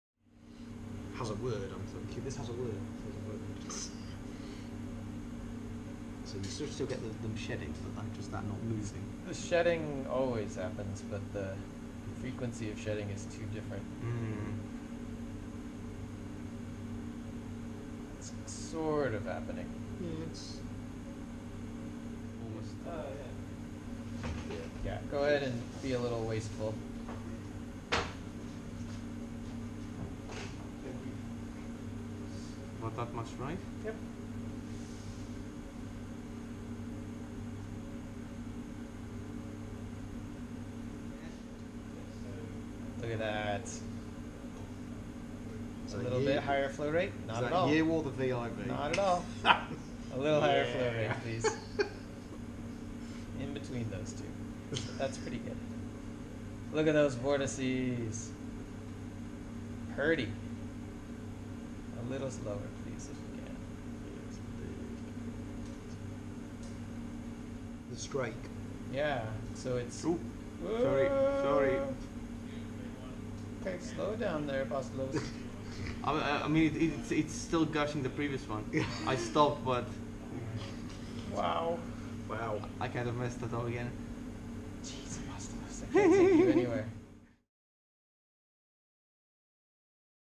Vortex Induced Vibrations demo
Vortex Induced Vibrations demo for the Offshore Engineering and Analysis course at University of Southampton. The geometry is changed in the last test by adding a helical strake which suppresses the vibrations completely.